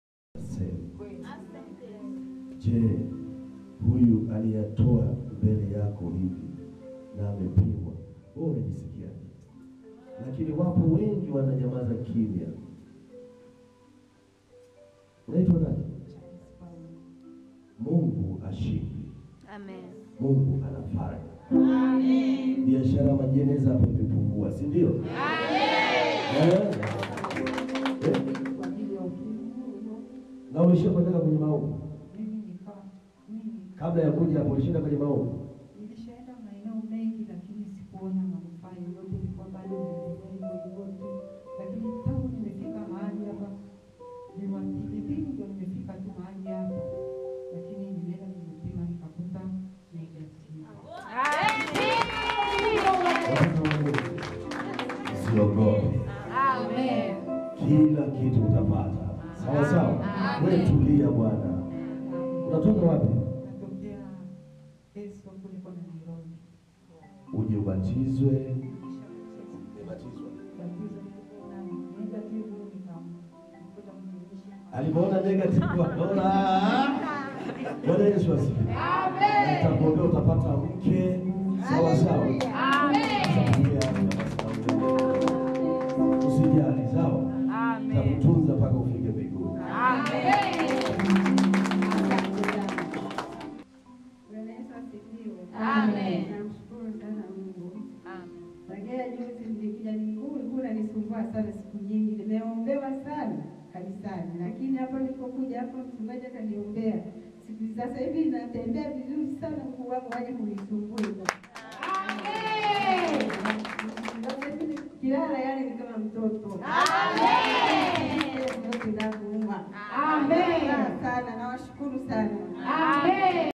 Aliyeponywa ukimwi pamoja na mwanaye katika kanisa la YESU NI BWANA NA MWOKOZI WA MATAIFA YOTE
Aliyekuwa monjwa wa ukimwi (HIV / AIDS)  akishuhudia jinsi yeye na mwanaye walivyoponywa baada ya kuombewa katika kanisa la YESU NI BWANA NA MWOKOZI WA MATAIFA YOTE.